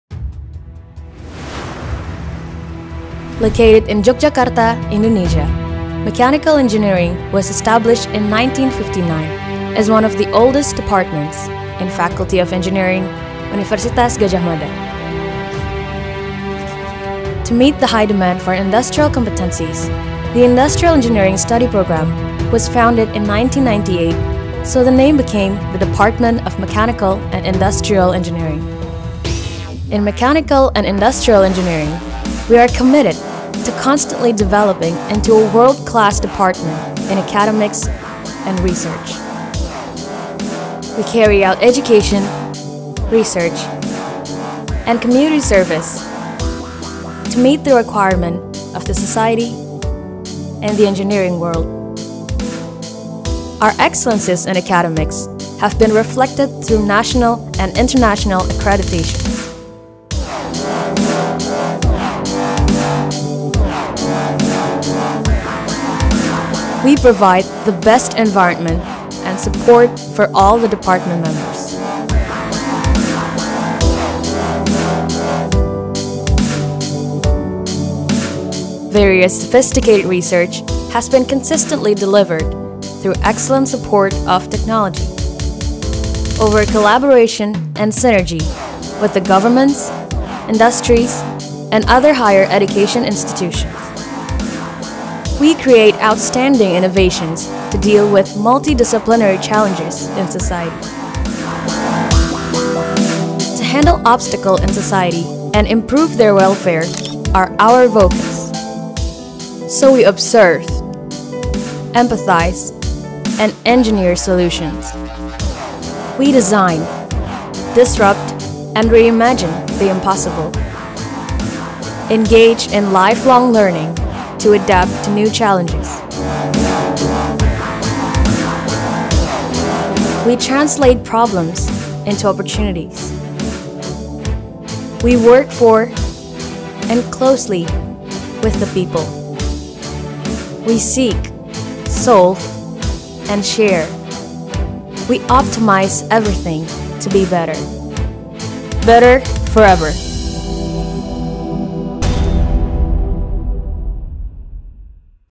DTMI Better Forever! [Short Profile in English]
Welcome to the Department of Mechanical and Industrial Engineering at Universitas Gadjah Mada (UGM), Yogyakarta, Indonesia.

The Department of Mechanical and Industrial Engineering is a highly-ranked department in the country having Undergraduate Program, Master Program and Doctoral Program in both Mechanical Engineering (ME) and Industrial Engineering (IE). Mechanical Engineering deals with the design of mechanical and thermal systems based on the principles of physics, mathematics and materials science whereas Industrial Engineering is related to the development, improvement, implementation and evaluation of integrated systems of equipment, material, process, information, money and people.

The Department has well-equipped laboratories, modern class rooms, a library and other facilities to support our educational program and research. Our faculty colleagues graduated from well recognized universities in the world (USA, Europe, Australia and Japan) and they are expert in their fields, innovative and dedicated to student success. The research activities of our faculty and students in the field of Mechanical Engineering Program include Applied Mechanics, Energy, Engineering Materials and Manufacturing whereas research activities in Industrial Engineering Program concern with Manufacturing Process and System, Production Engineering, Operation Research and Ergonomics. These Department’s research activities have significant impact on our society and scientific community.

Reports of tracer study show that our alumni have successful careers in industry, enterpreneurship, academia, national laboratories, government and non-profit organizations and they help to maintain the Department at the forefront of education and research in mechanical and industrial engineering.

Please enjoy your visit to the Department of Mechanical and Industrial Engineering.

DEPARTMENT OF MECHANICAL AND
INDUSTRIAL ENGINEERING
Universitas Gadjah Mada
Jl. Grafika No. 2, Yogyakarta 55281, Indonesia